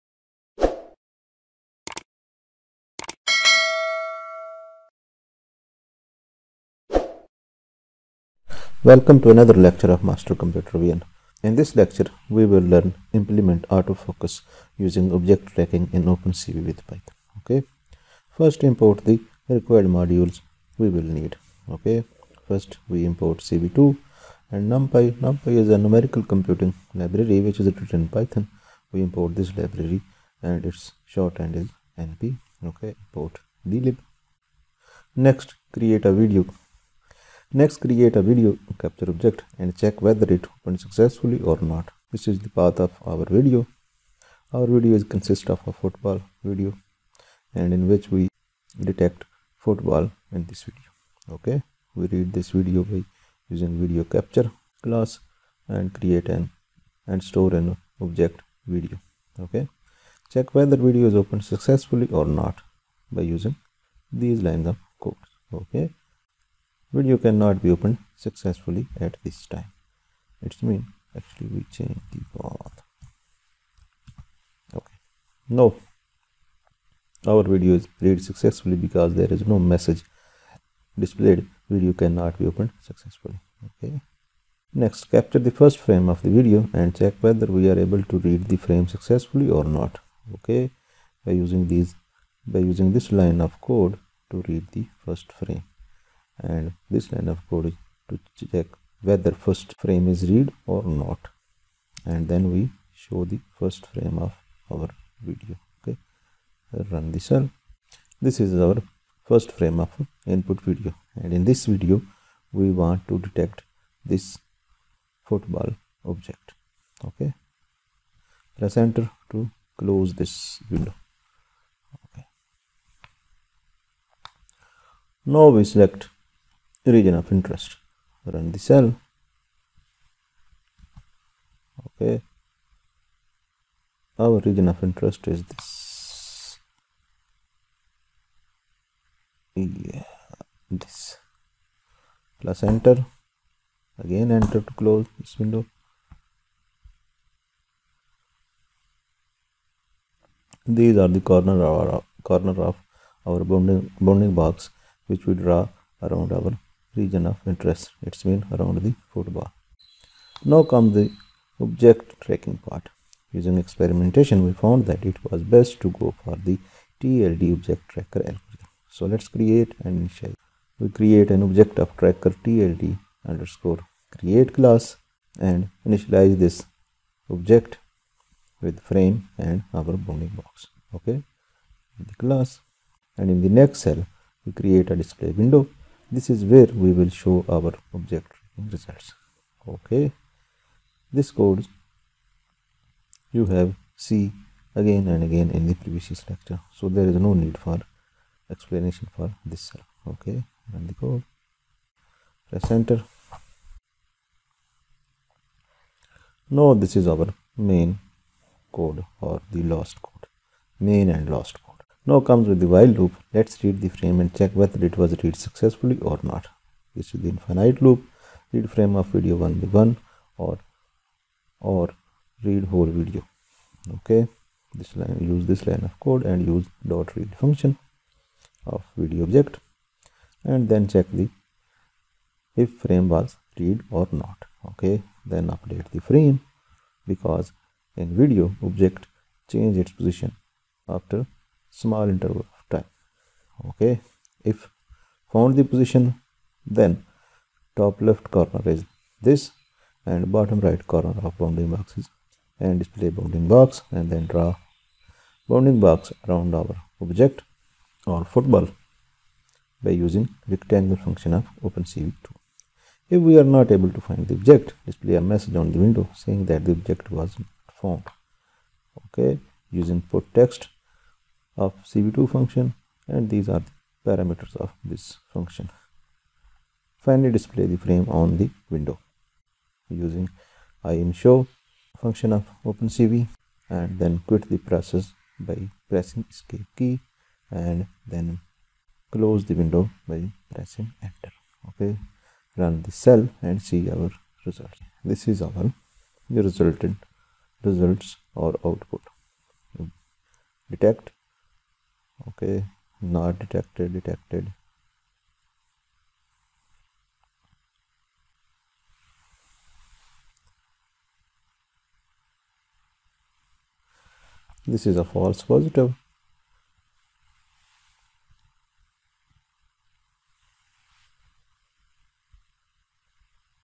Football Tracking in a Video Using Object Tracking In OpenCV with Python
In this Lecture, we will implement an autofocus utility that can be used
in football to track the ball throughout the game.